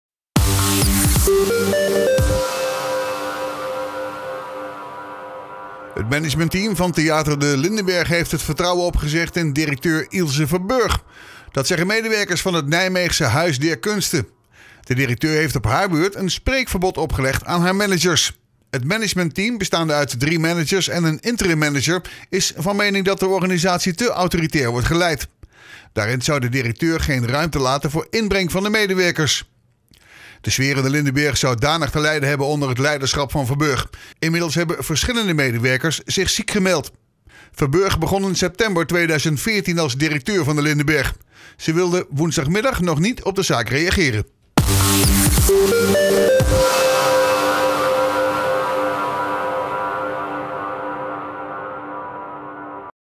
Lindenberg: 'management op non-actief, vertrouwen in directeur opgezegd'
NIJMEGEN – Het managementteam van theater De Lindenberg heeft het vertrouwen opgezegd in directeur Ilse Verburgh. Dat zeggen medewerkers van het Nijmeegse Huis der Kunsten. De directeur heeft op haar beurt een spreekverbod opgelegd aan haar managers. Ook is het managementteam op non-actief gesteld. De Raad van Toezicht van de Lindenberg staat wel achter de directeur.